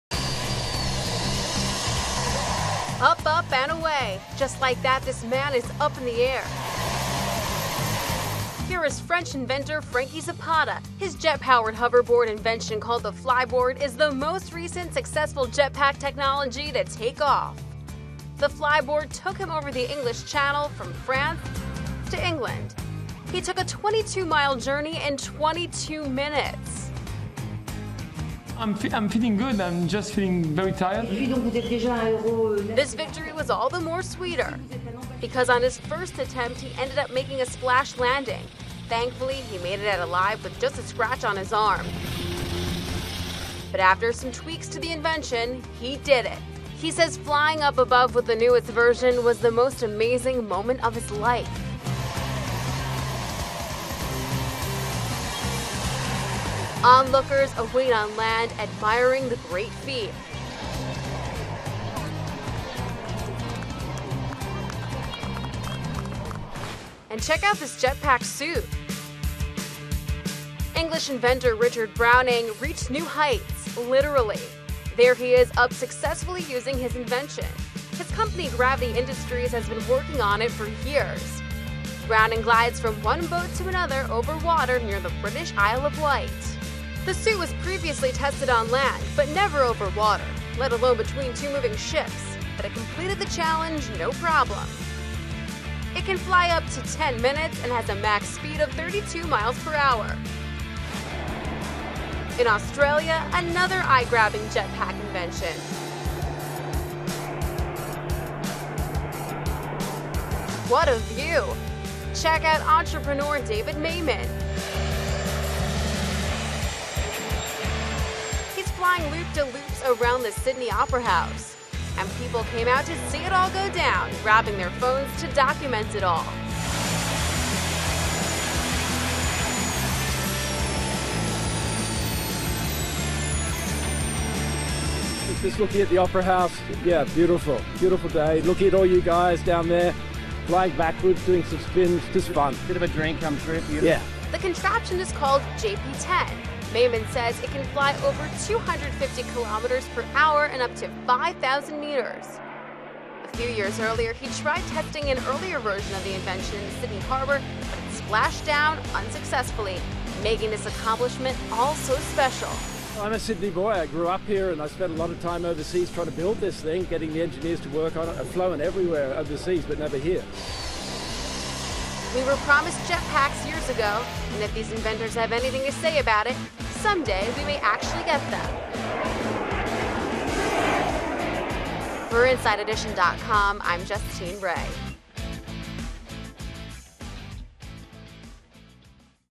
Could Jetpacks Be the New Way to Travel?
Could jetpacks be the new way to travel? Inventors across the globe are trying to develop the tech. The most recent successful attempt was in France by inventor Franky Zapata. He flew over the English Channel. In the United Kingdom, Richard Browning invented a jetpack suit that successfully allowed him to travel from one boat to another. Down in Australia, David Mayman flew in circles around the Sydney Opera House. InsideEdition.com's Justine Re reports.